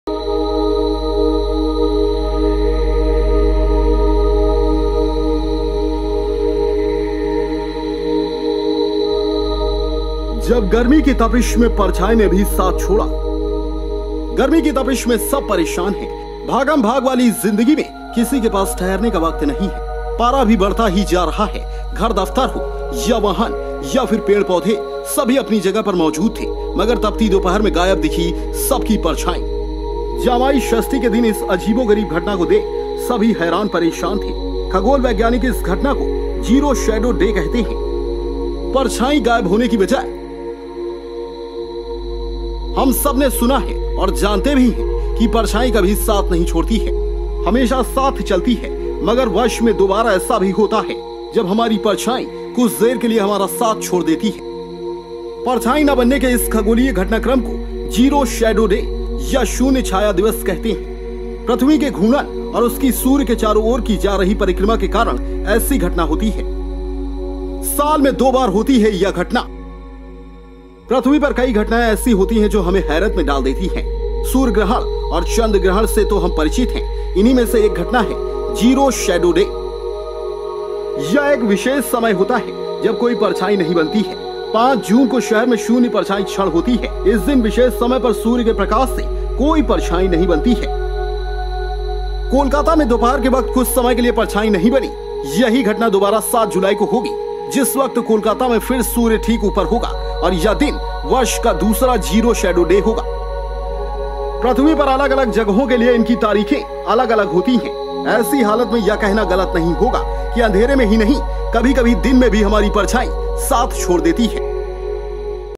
तपती दोपहर में गायब दिखी सबकी परछाई
गर्मी की तपिश में सब परेशान हैं। भागमभाग वाली जिंदगी में किसी के पास ठहरने का वक्त नहीं है। पारा भी बढ़ता ही जा रहा है। घर-दफ्तर हो या वाहन या फिर पेड़ पौधे सभी अपनी जगह पर मौजूद थे। मगर तपती दोपहर में गायब दिखी सबकी परछाई। जामाई षष्ठी के दिन इस अजीबो गरीब घटना को देख सभी हैरान परेशान थे। खगोल वैज्ञानिक इस घटना को ‘जीरो शैडो डे’ कहते हैं।
परछाई गायब होने की वजह
हम सबने सुना है और जानते भी हैं क‌ि परछाई कभी साथ नहीं छोड़ती है, हमेशा साथ चलती है। मगर वर्ष में दो बार ऐसा भी होता है जब हमारी परछाई कुछ देर के लिए हमारा साथ छोड़ देती है। परछाई न बनने के इस खगोलीय घटनाक्रम को ‘जीरो शैडो डे’ या शून्य छाया दिवस कहते हैं। पृथ्वी के घूर्णन और उसकी सूर्य के चारों ओर की जा रही परिक्रमा के कारण ऐसी घटना होती है।